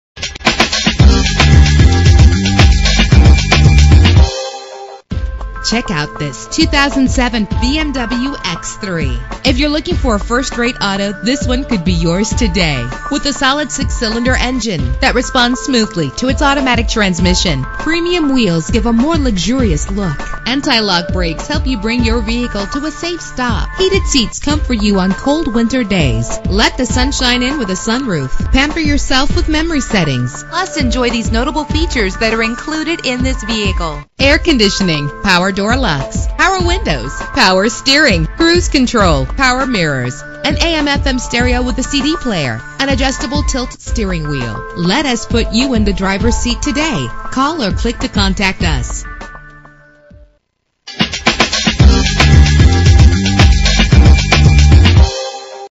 2007 BMW X3 - Sumner WA
Year: 2007
Make: BMW
Model: X3
Trim: 3.0si
Engine: 3.0 liter inline 6 cylinder 24 valve
Transmission: Automatic
Color: Black
Mileage: 41836
Address:
16302 Auto Lane